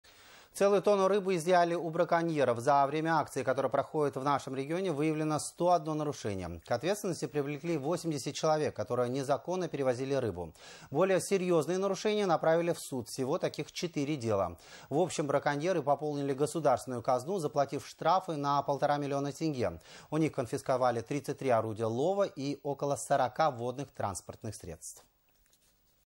В Атырау у браконьеров изъято около 1 тонны рыбы
Целую тонну рыбы изьяли у браконьеров. За время акции которая проходит в нашем регионе выявлено 101 нарушение. К ответственности привлекли 80 человек, которые незаконно перевозили рыбу. Более серьезные нарушения направили в суд, всего таких четыре дела. В общем браконьеры пополнили государственную казну заплатив штрафы на полтора миллиона тенге. У них конфисковали  33 орудия лова и около 40 водных транспортных средств.